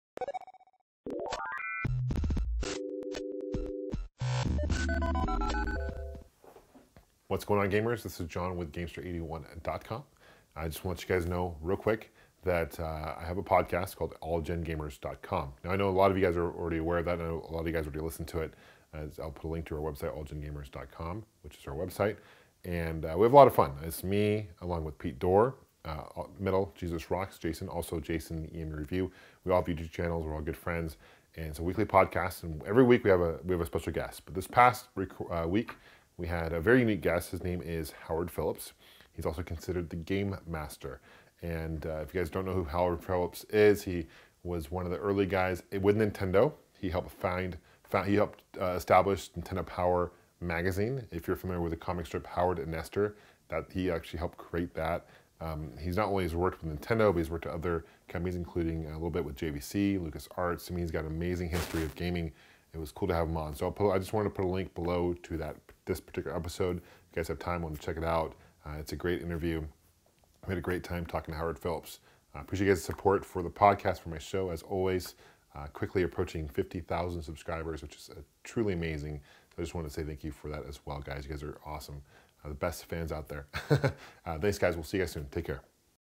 Howard Phillips on All Gen Gamers! - Gamester81
Howard Philips who is one of the key guys behind the original Nintendo Power Magazine and other key Nintendo initiatives back in the 1980's/1990's is a guest on my video game podcast called All Gen Gamers.

We're also on iTunes!